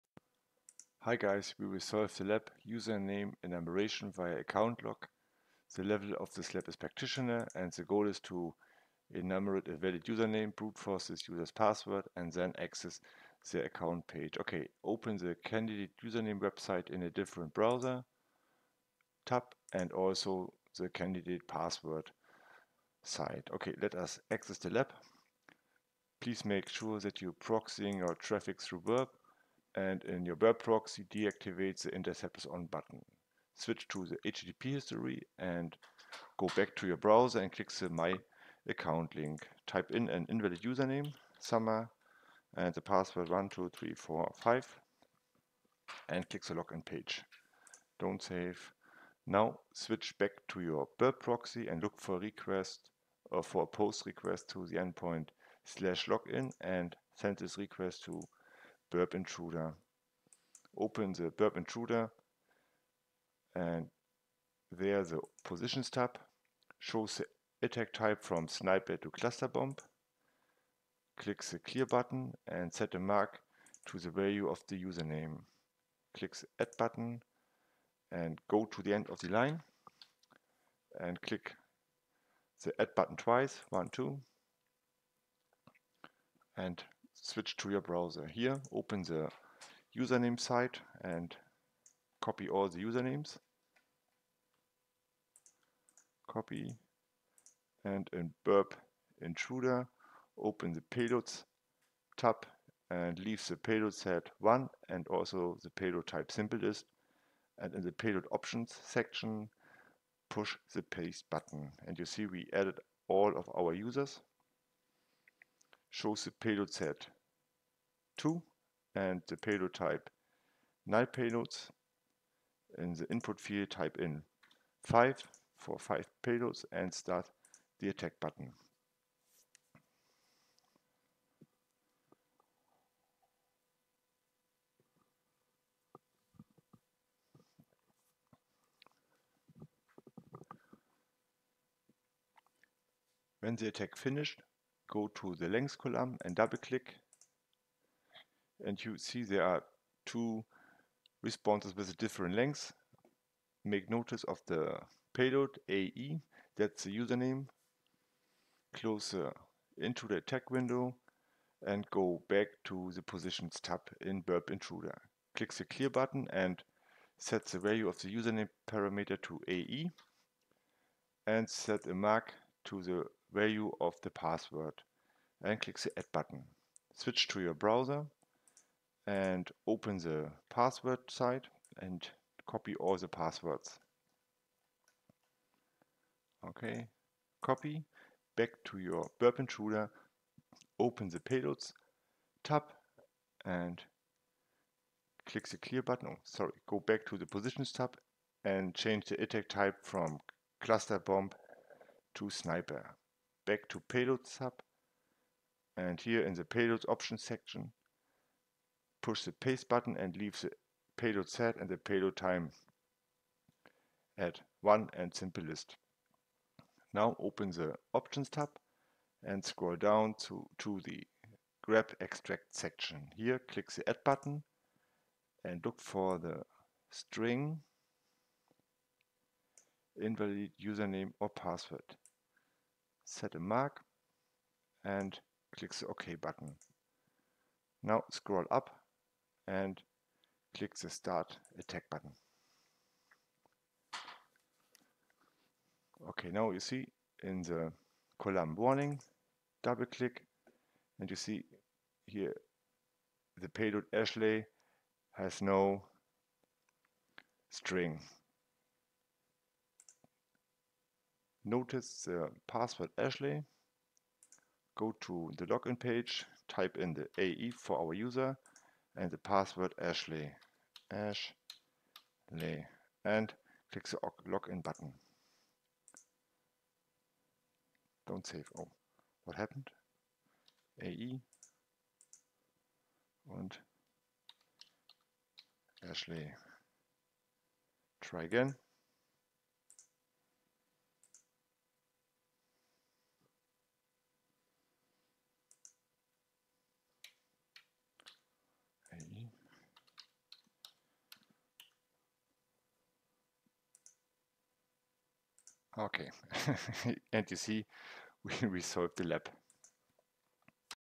Username enumeration via account lock (Video solution, Audio)
This video shows the lab solution of "Username enumeration via account lock" from Web Security Academy (Portswigger)